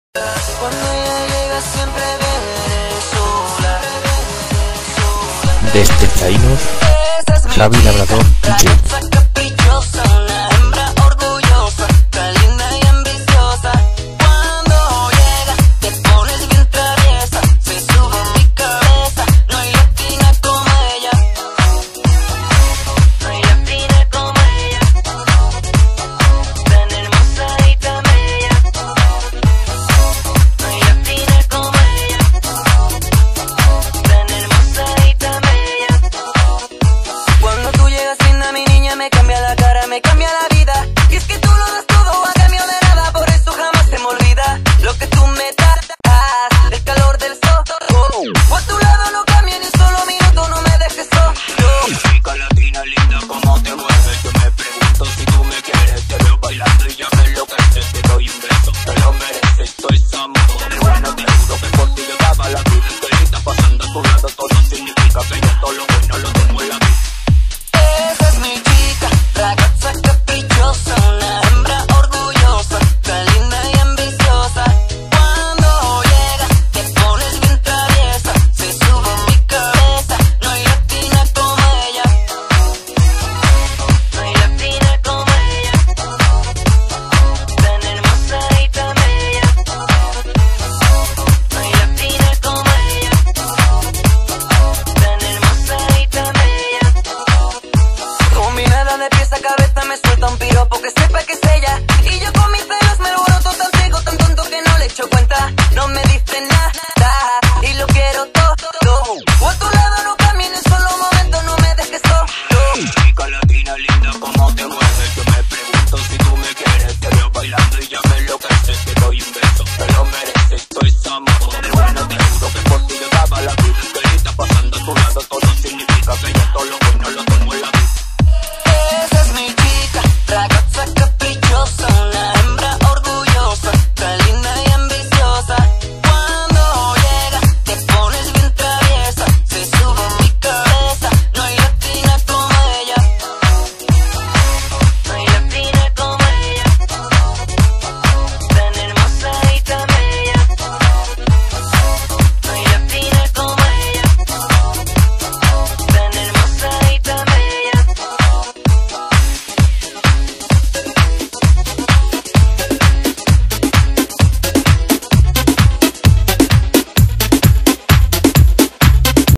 09 ESPECIAL NAVIDAD 2012 Javi Labrador Dj
Para desear a todo el mundo una FELIZ NAVIDAD aquí teneis la nueva sesion BIENVENIDOS A LA FIESTA - Especial Navidad 2012.